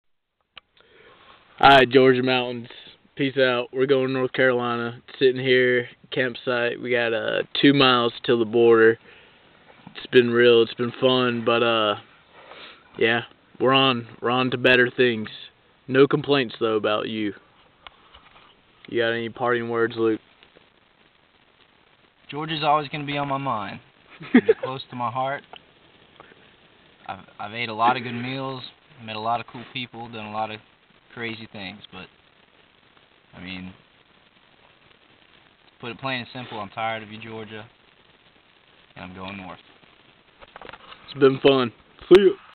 Goodbye, Georgia! (take 2)
We say goodbye to a place we love again.